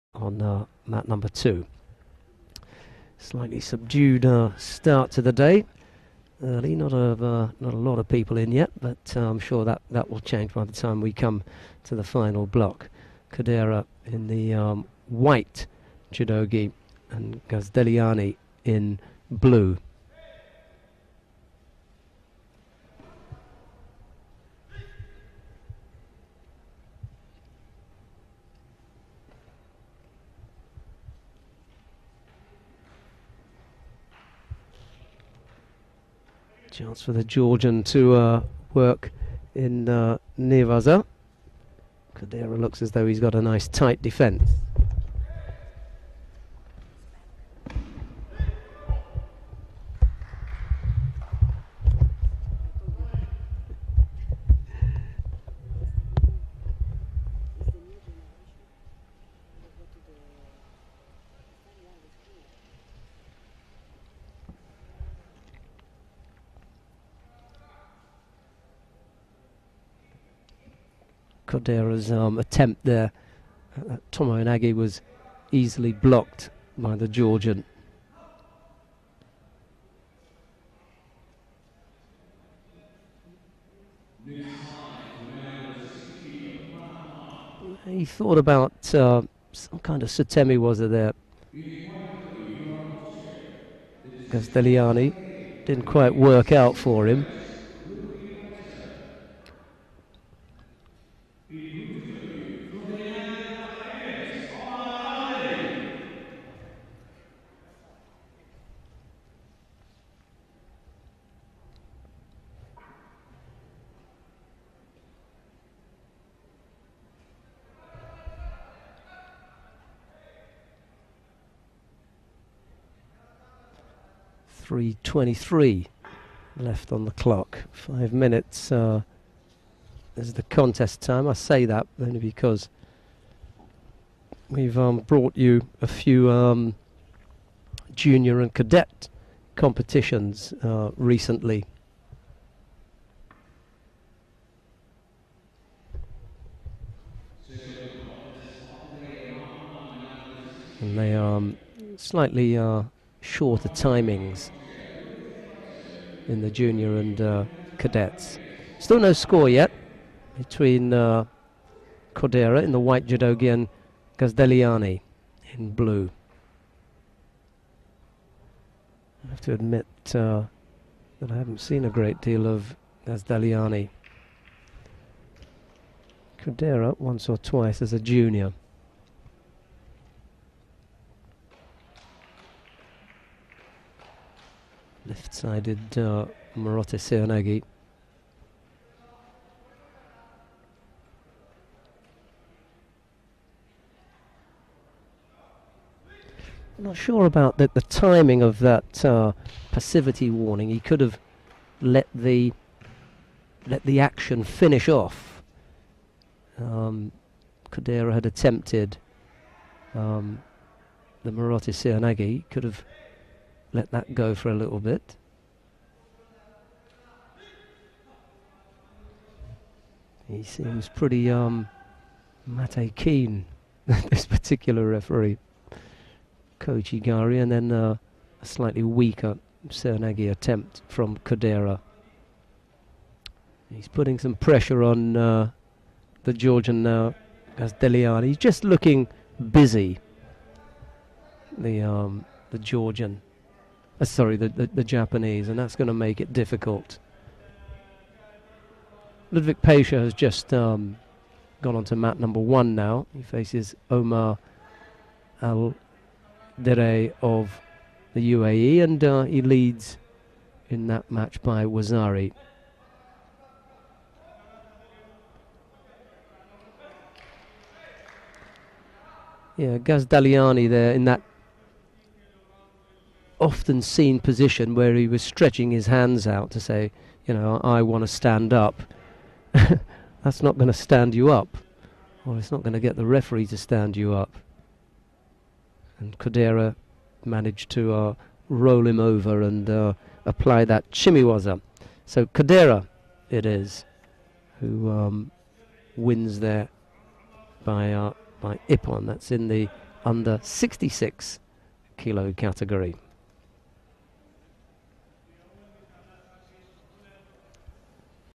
Judo Grand Prix Abu Dhabi 2012: Kodera (JPN) - Gazdeliani (GEO) [-66kg]
Masashi Kodera (JPN) - Jakob Gazdeliani (GEO) [-66kg]
Judo Grand Prix Abu Dhabi 2012